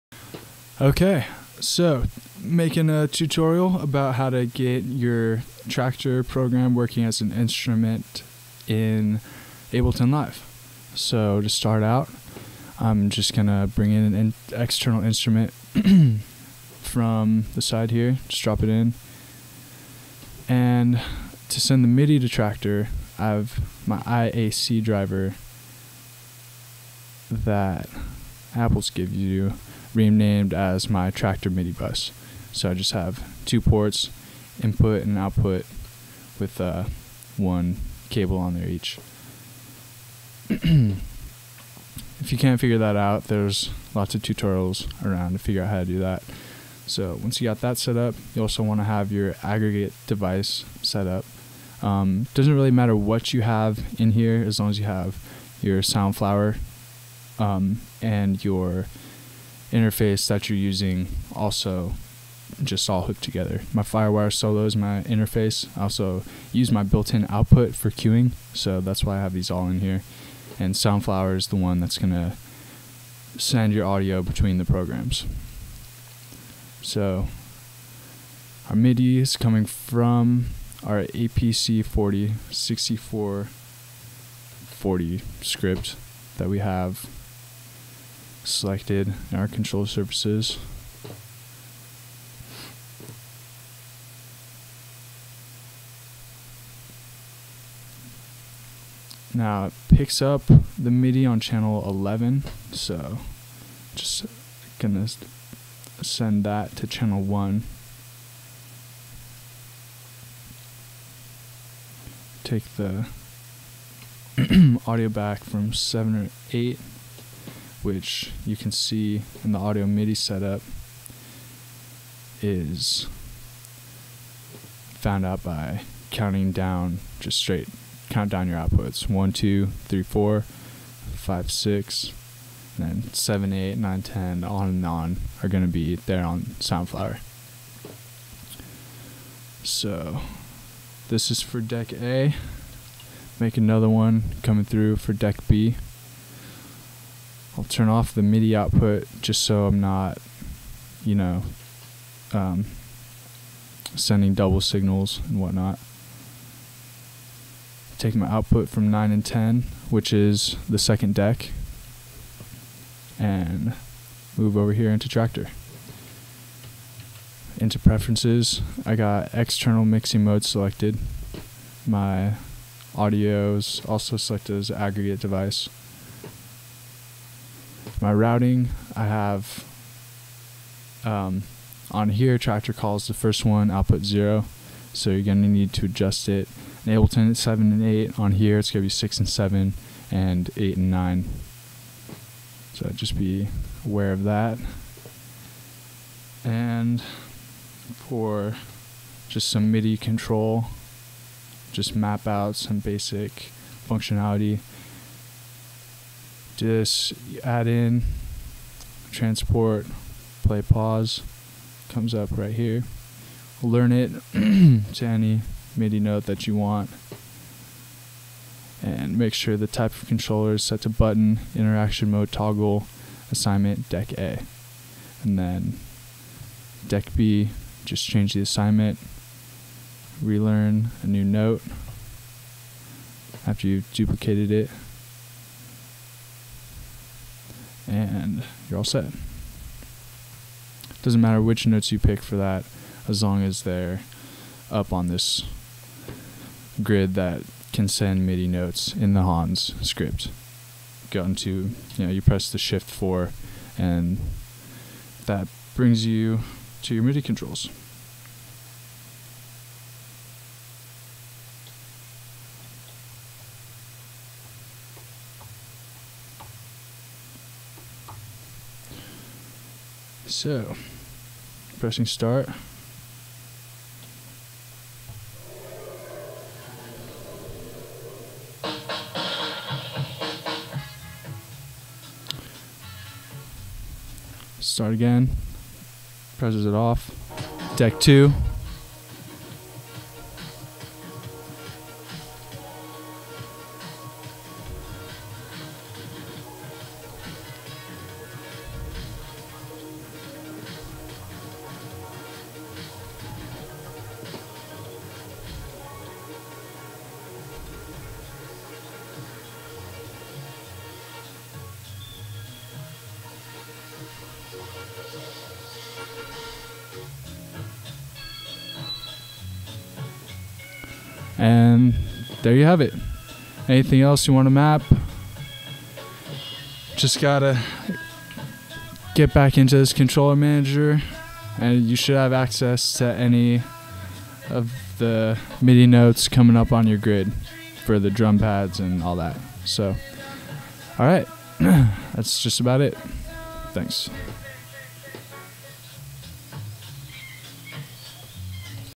Tutorial: How to use Traktor and Ableton With APC-40
This is my first tutorial, I've been trying to figure out a way to use traktor as the decks in my live set, without adding anymore extra controllers besides my APC. This tutorial uses two add on programs that can both be found for free, Soundflower and the Hanz APC 64_40 Remote Script. I'm also using Apple's Aggregate Device feature and IAC Driver, both found in Audio Midi Setup. Sorry, my English not so good.